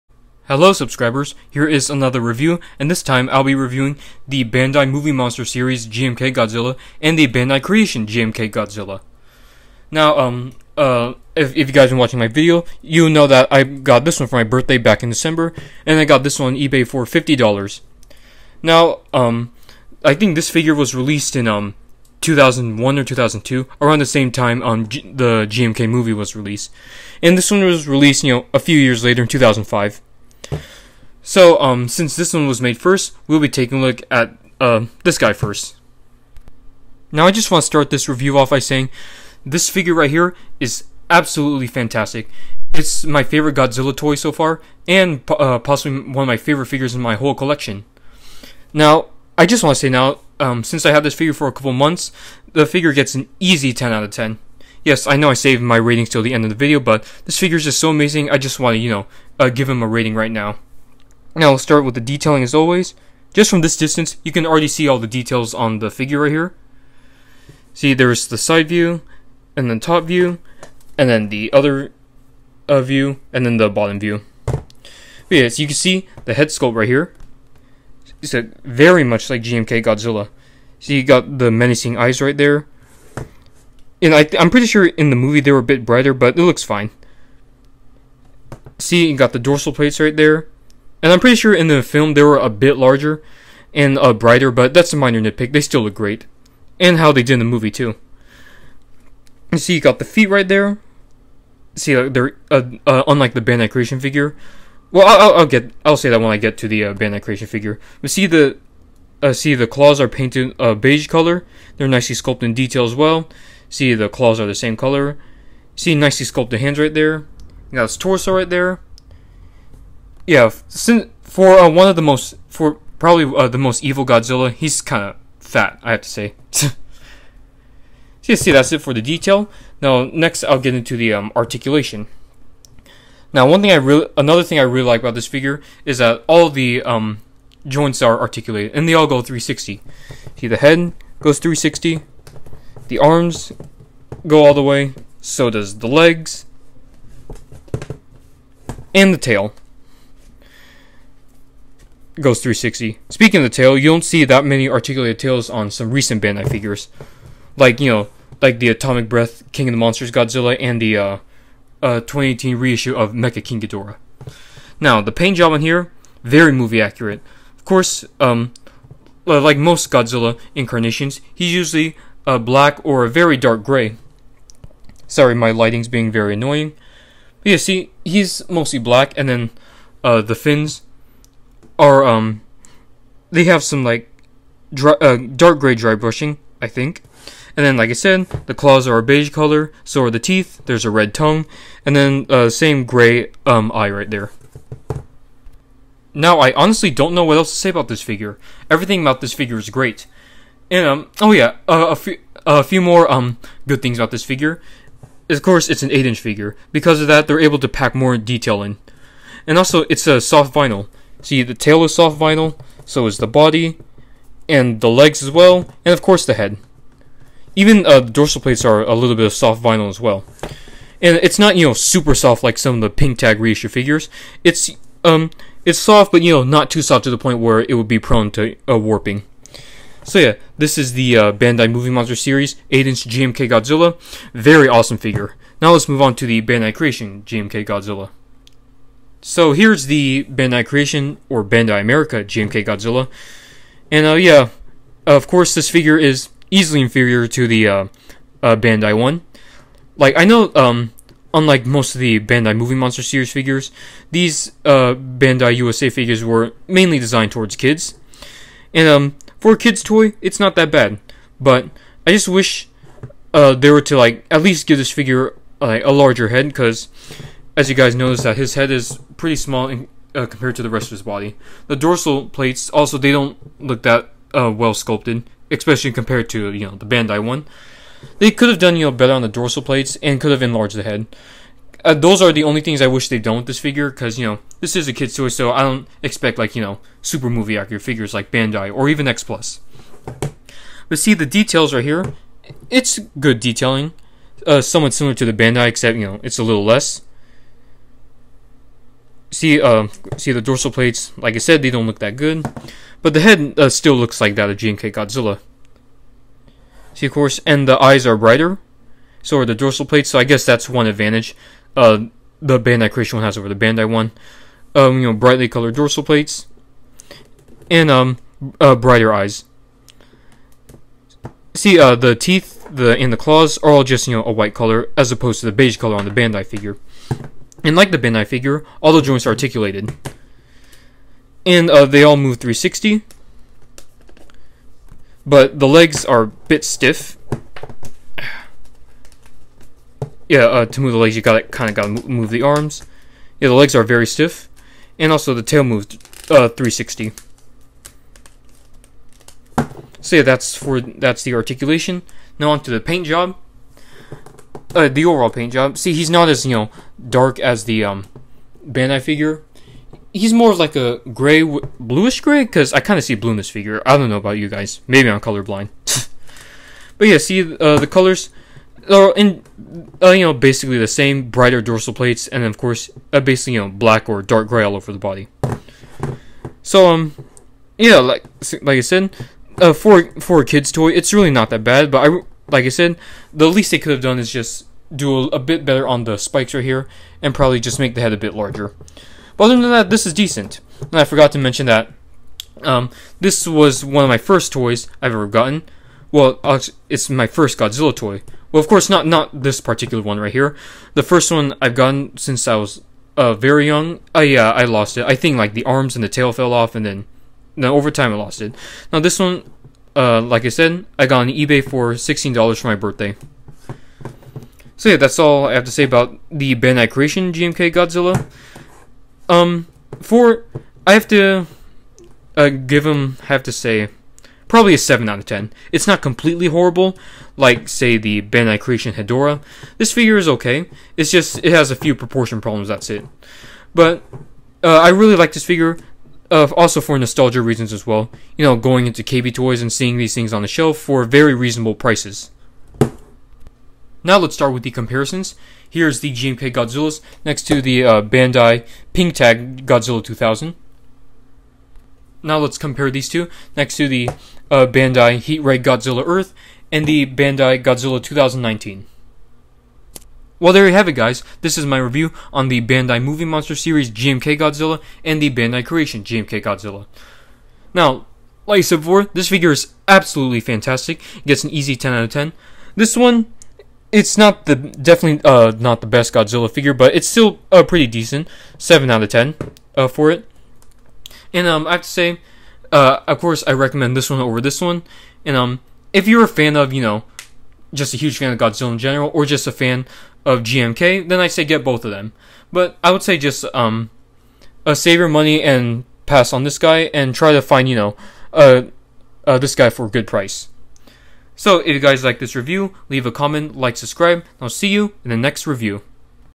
Bandai Movie Monster Series & Bandai Creation GMK Godzilla figures review (+13)
My review on Bandai and Bandai Creation’s versions of the 2001 Godzilla.
Bandai: 10/10
Bandai Creation: 7/10

For viewers +13